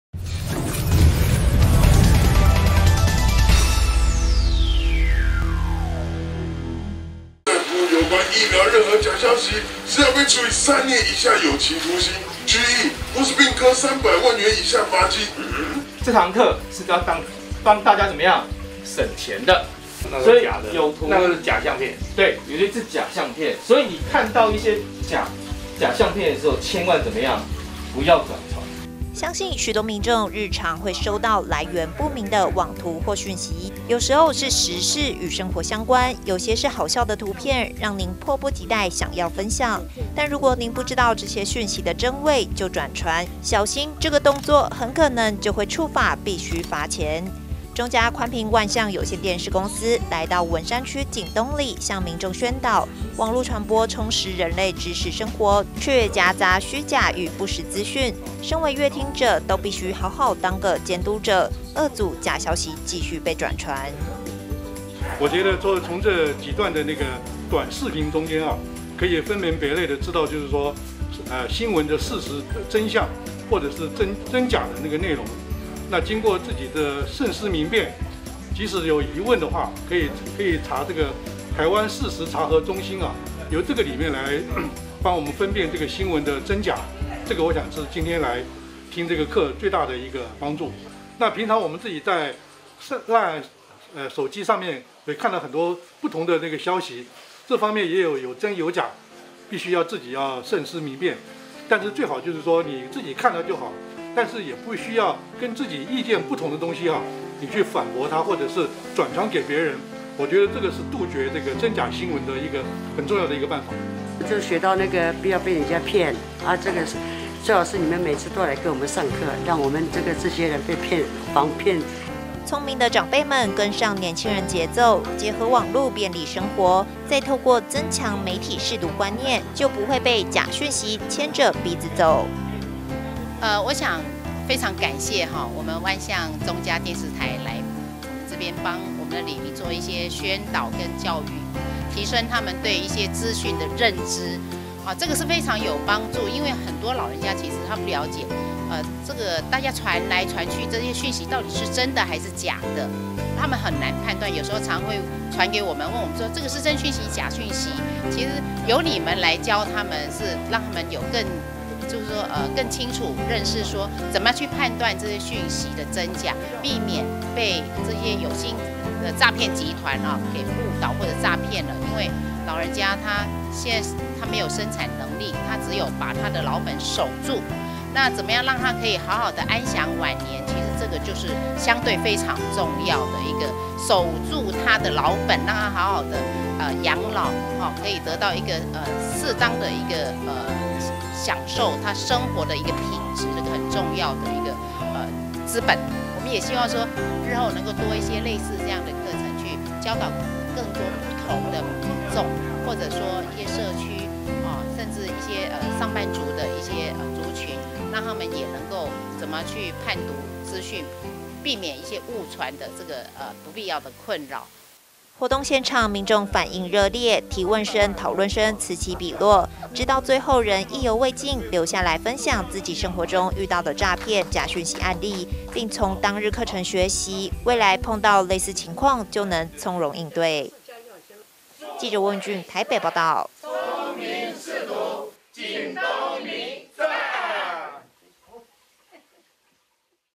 媒體識讀｜思辨再三防詐騙 聰明監督者就是你
中嘉寬頻旗下萬象有線電視善盡地方媒體責任，除了採訪新聞深耕在地，也舉辦媒體識讀講座，這次來到文山區景東里，透過淺顯易懂的防詐騙動畫，教導長輩思辨能力，辨別訊息真偽。